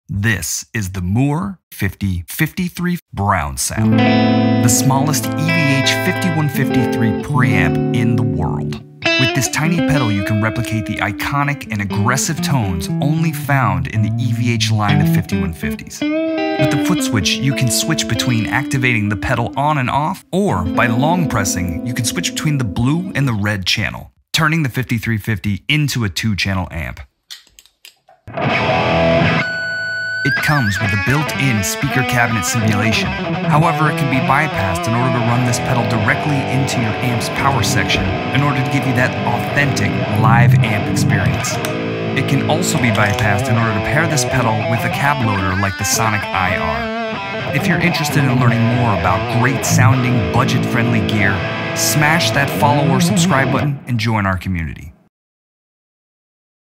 Tiny Pedal… HUGE HIGH GAIN TONE! ⚡️
The Mooer Fifty Fifty-Three Brown Sound Pre-Amp Guitar Pedal is a must-have for any guitar player looking to add some flavor to their sound!

With its warm, rich tones and authentic tube amp emulation, this pedal is perfect for players who want to add some classic EVH character to their playing.

Whether you're playing rock, blues, or METAL, the Fifty Fifty-Three has you covered. Its intuitive controls make it easy to dial in your perfect sound, and its compact size makes it perfect for gigging musicians.

So if you're looking for a pedal that will help you get that EVH tone you've been chasing but in a small form factor, the Mooer Fifty Fifty-Three is the perfect choice!

⭐️ SUPPORT MAD MAN MUSIC BY:
🙏🏼 Giving  "Super Thanks" 🤩